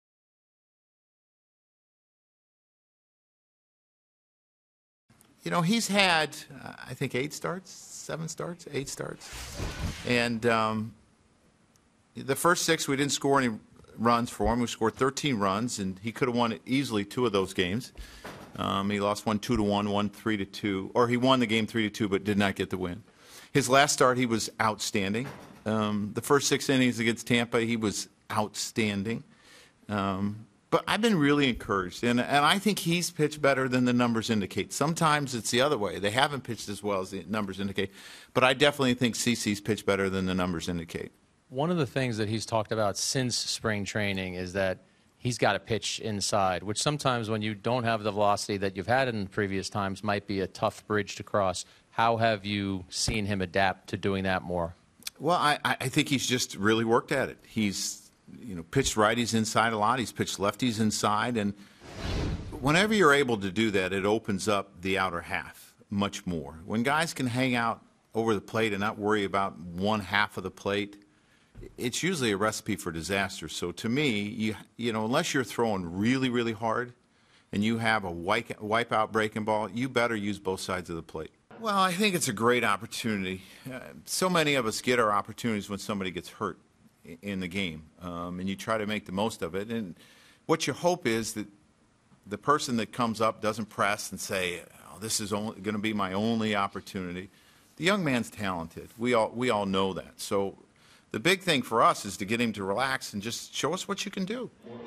Joe Girardi on Slade Heathcott & CC Sabathia
Hear from Joe Girardi on Slade Heathcott & CC Sabathia.